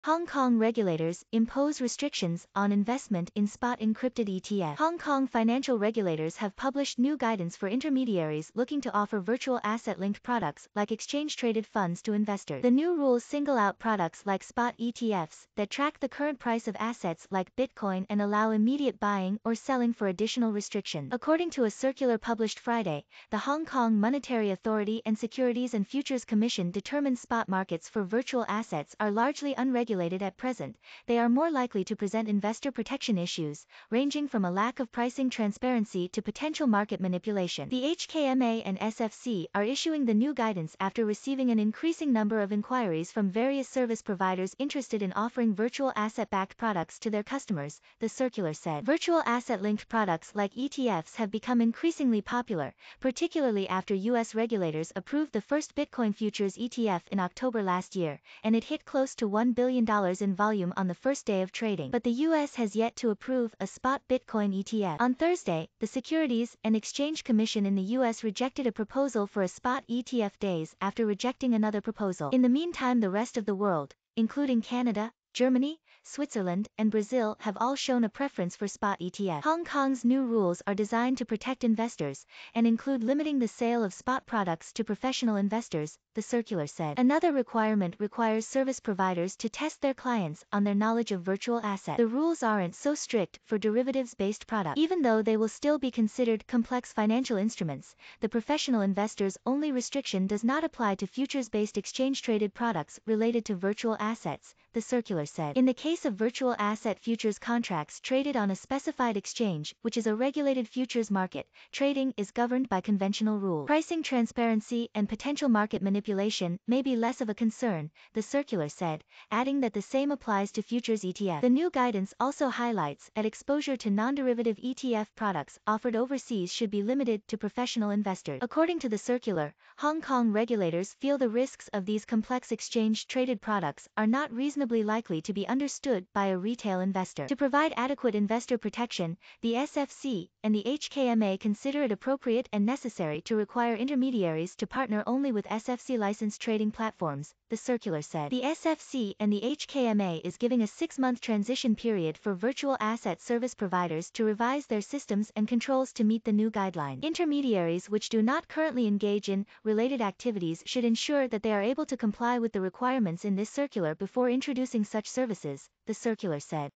Hong Kong regulators impose restrictions on investment in spot encrypted ETF
Hong Kong regulators impose restrictions on investment in spot encrypted ETF Hong Kong financial regulators have published new guidance for intermediaries looking to offer virtual asset-linked products like exchange-traded funds  to investors. The new rules single out products like spot ETFs that track the current price of assets like bitcoin and allow immediate buying or selling for additional restrictions. According to a circular published Friday the circular said. The SFC and the HKMA is giving a six-month transition period for virtual asset service providers to revise their systems and controls to meet the new guidelines. adding that the same applies to futures ETFs. The new guidance also highlights that exposure to non-derivative ETF products offered overseas should be limited to professional investors. According to the circular the circular said. Another requirement requires service providers to test their clients on their knowledge of virtual assets. The rules aren’t so strict for derivatives-based products. Even though they will still be considered complex financial instruments Switzerland and Brazil have all shown a preference for spot ETFs. Hong Kong’s new rules are designed to protect investors they are more likely to present investor protection issues In the case of virtual asset futures contracts traded on a specified exchange which is a regulated futures market the circular said. Virtual asset-linked products like ETFs have become increasingly popular and include limiting the sale of spot products to professional investors trading is governed by conventional rules. Pricing transparency and potential market manipulation may be less of a concern